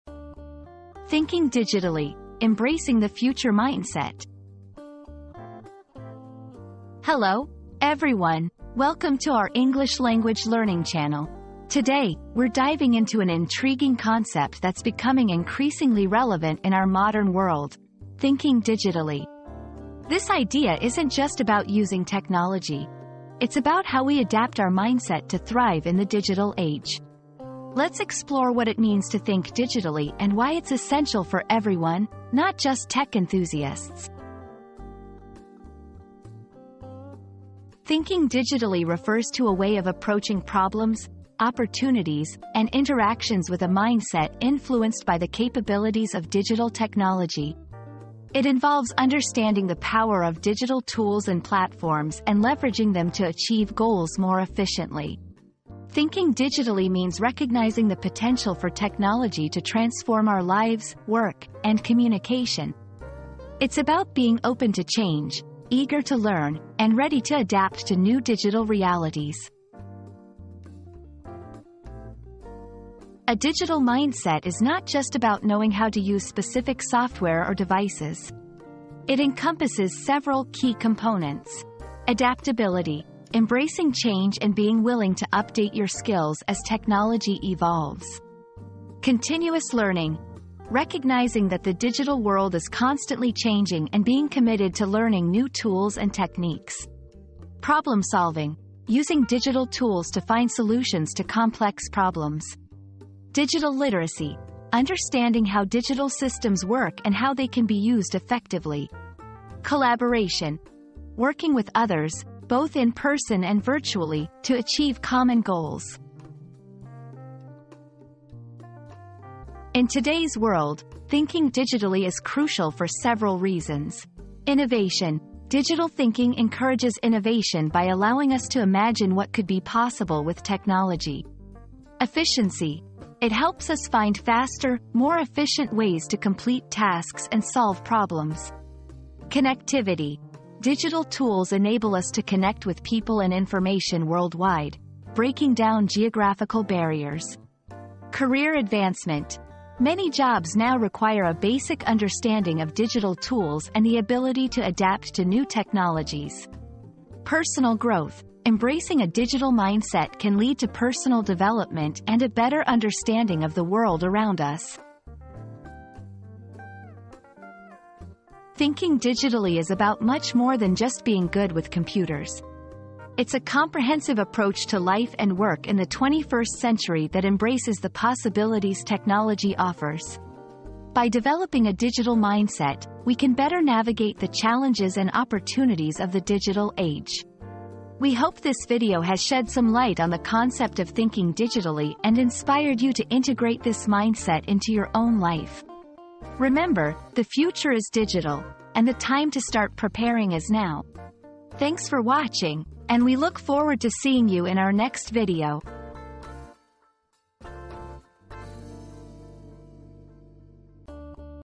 Thinking Digitally: Embracing the Future Mindset
Embrace the Future Mindset: Thinking Digitally • Unlock the power of digital thinking and embrace the future mindset in this captivating video. Discover how to adapt and thrive in the ever-evolving digital landscape.

00:00 • Introduction - Thinking Digitally: Embracing the Future Mindset
00:37 • What Does "Thinking Digitally" Mean?
01:12 • Key Components of a Digital Mindset
01:59 • Why is Thinking Digitally Important?